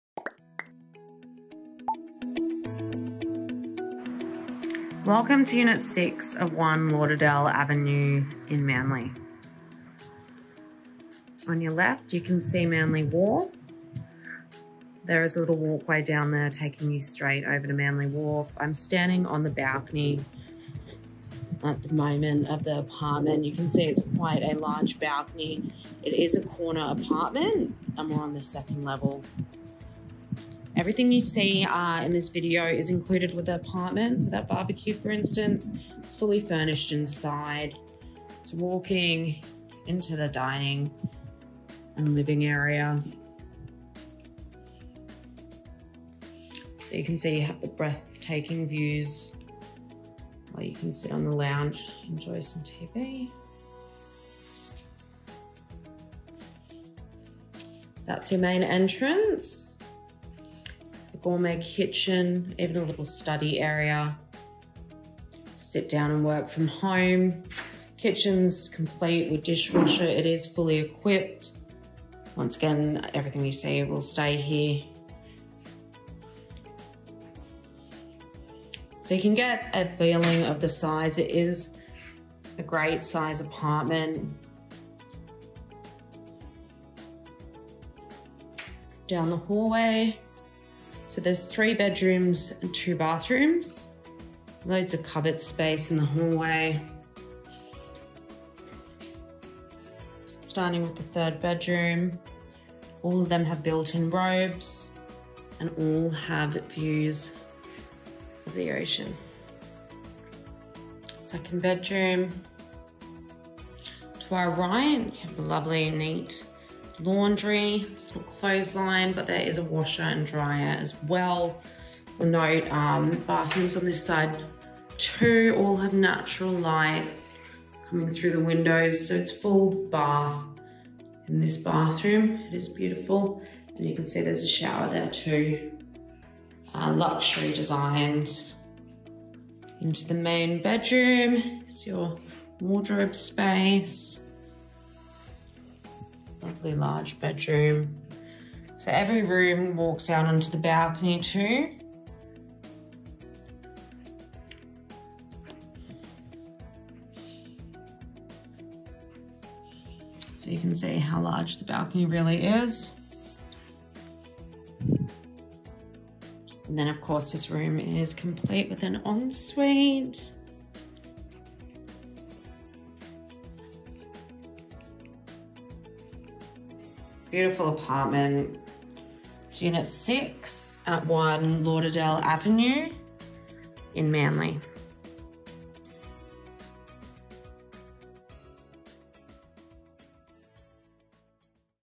Aquamarine Executive Apartment in Fairlight
Huge and bright three bedroom, two bathroom lower floor executive apartment in Fairlight's most sought after building. An excellent accommodation option for executives and families alike.